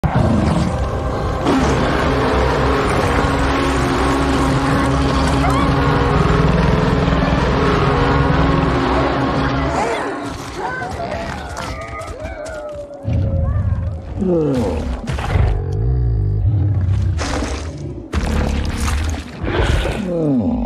Godzilla X Kong: The New Empire Kong Vs Wart Dogs With Red Blood
Little color change to the wart dogs' blood when kong fights them.

Just a short test compilation of animations I did the last few days after getting out of the hospital